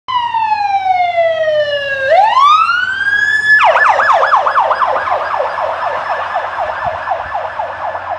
LBFD Rescue 11 Transporting
LBFD Engine and Rescue 11
Both transporting to Long Beach  memorial .